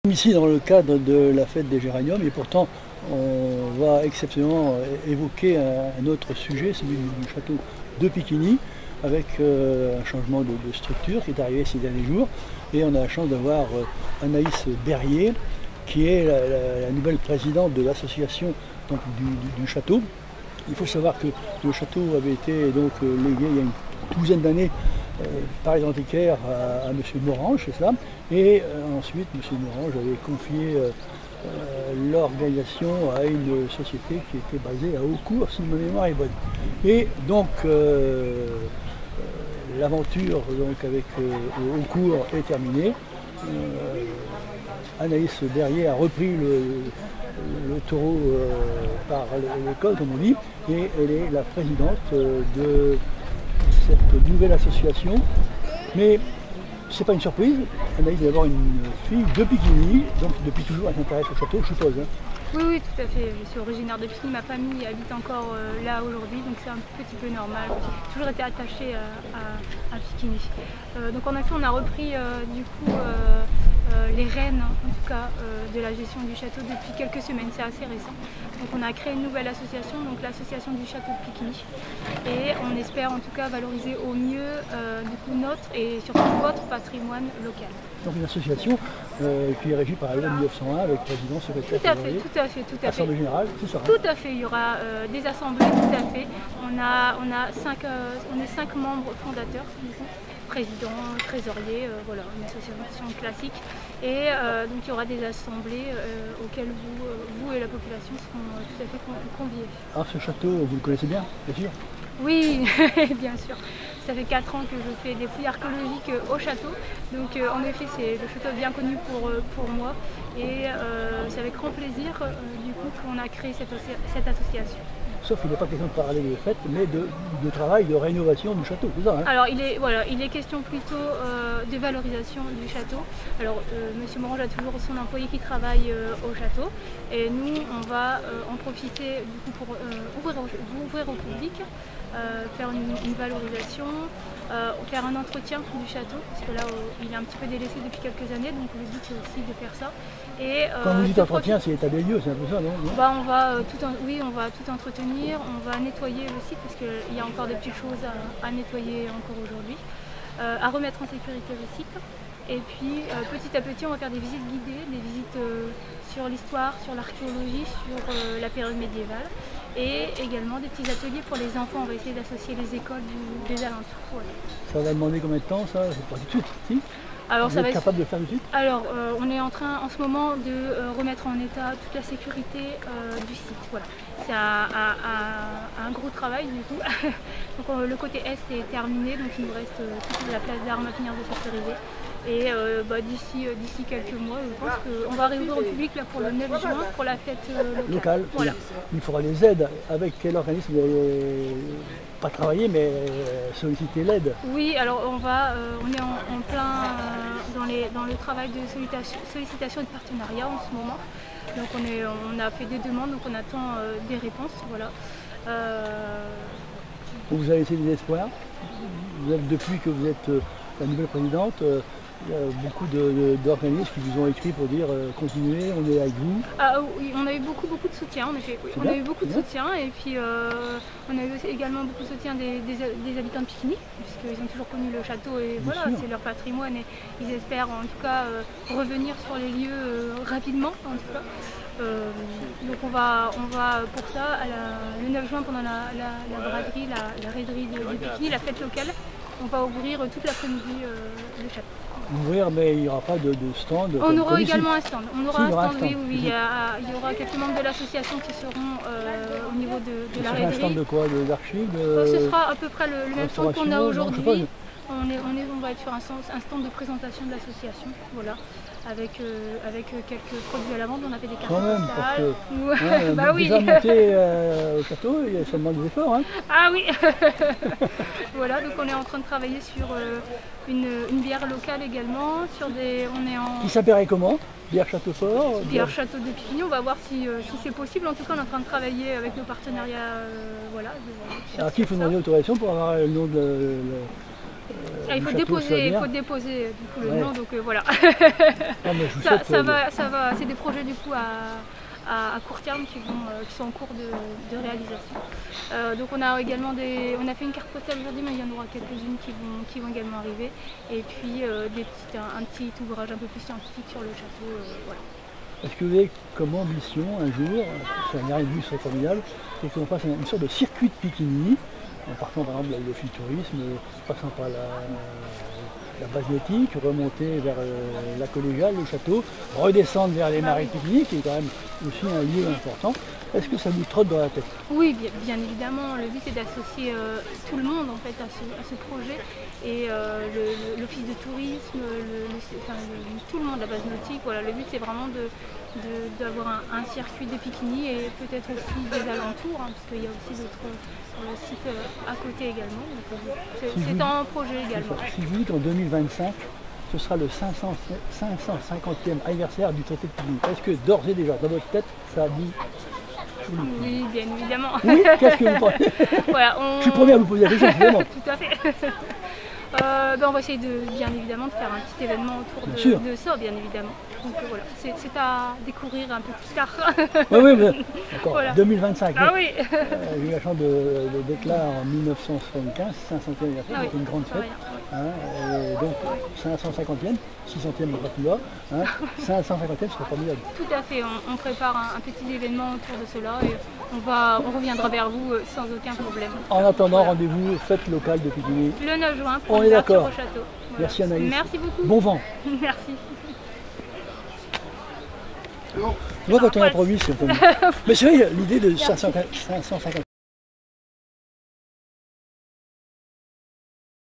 Picquigny - Anaïs Berrier à la tête de l'Association du château de Picquigny
Ainsi que nous l'avons signalé voici un mois, le chateau de Picquigny ne change pas de propriétaire mais de locataire. Le précédent n'était plus en odeur de sainteté et il a décidé de quitter les lieux.
Pour le remplacer et en plein accord avec la municipalité de Picquigny, une association a été créée.
Cette structure sera présidée par Anaïs Berrier une enfant du village et qui connait bien les lieux.
Récemment, lors de la Fête des Geraniums, nous avons rencontré Anaïs Berrier qui nous a expliqué ses objectifs (voir notre video)